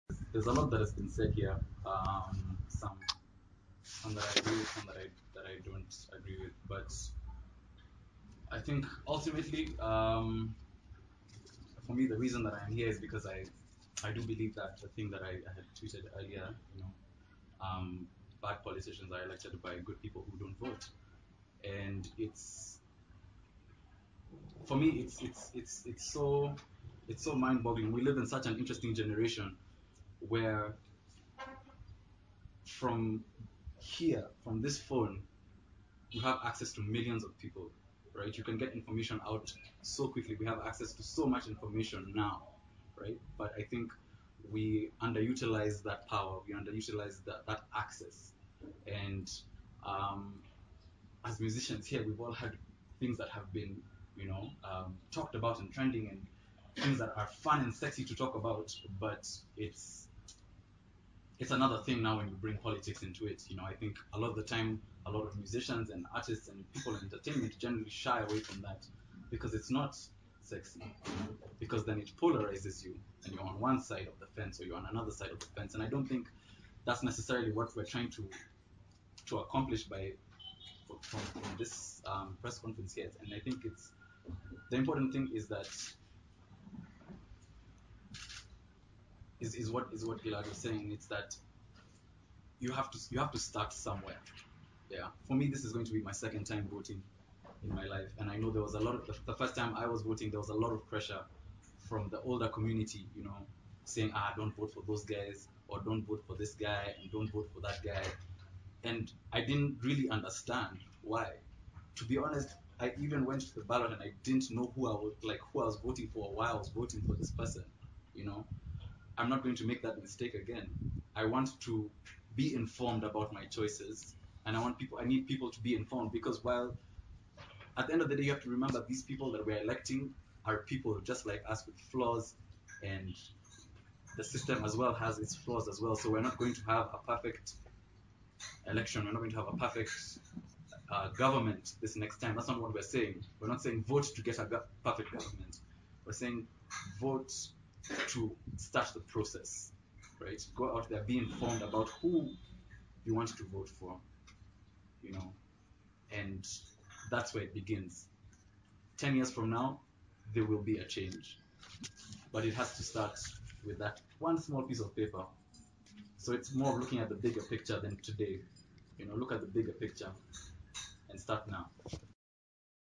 Kagwe Mungai Urges The Youth To Play Their Part In The Society Wisely
Kagwe Mungai speaks to the youth on the importance of voicing out their issues and voting wisely.